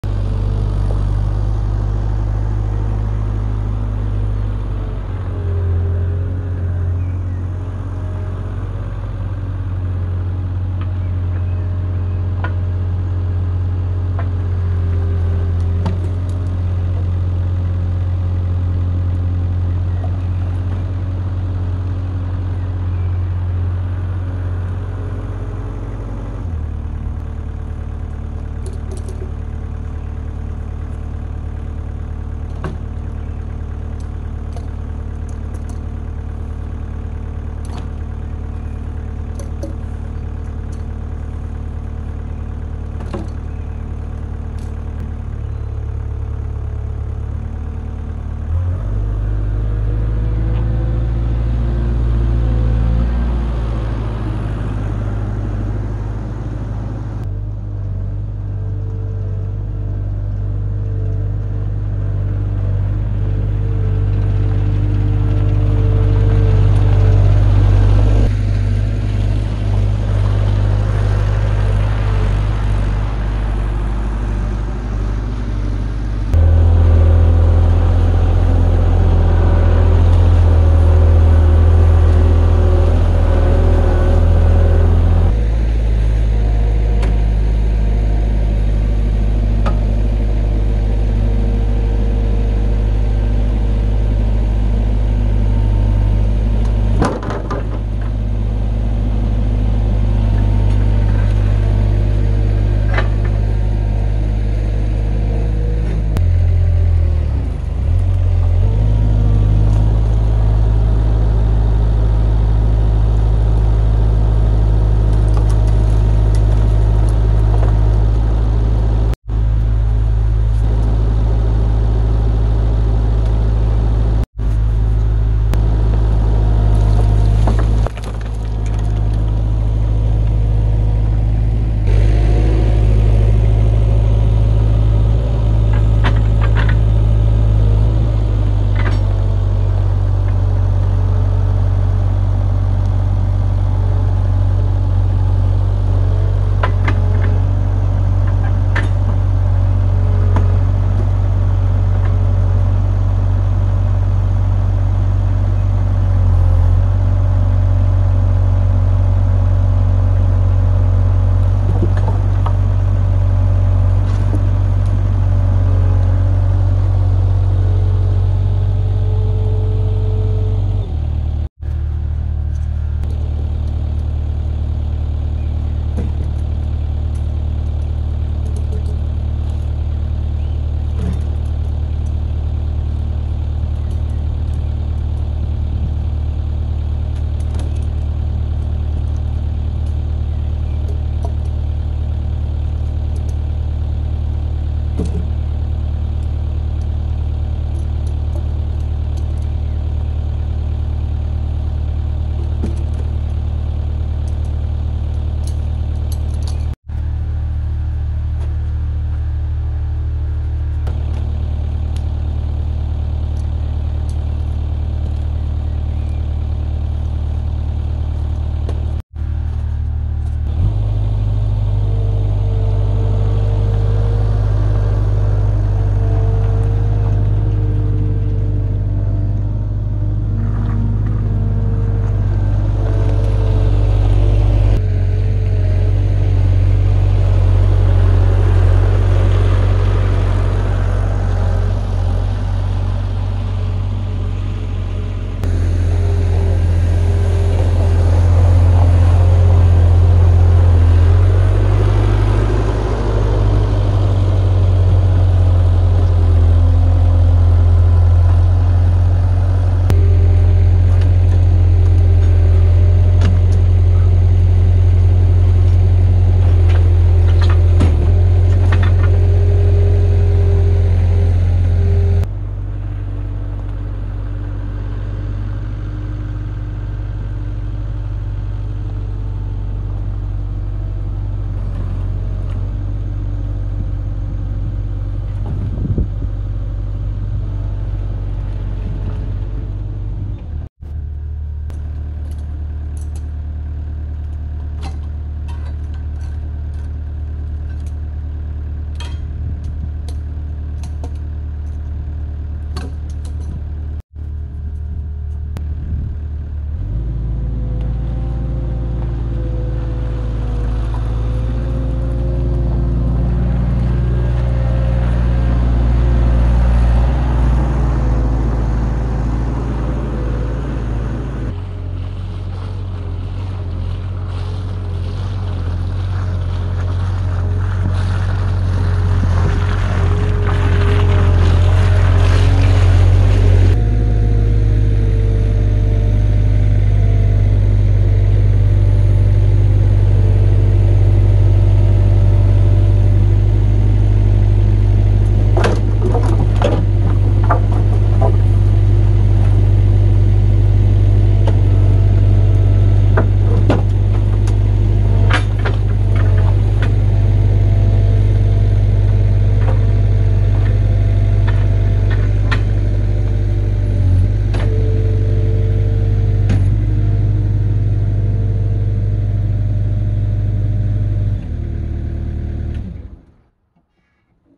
Avant 635 forklifting firewood logs
1.5m alder wood logs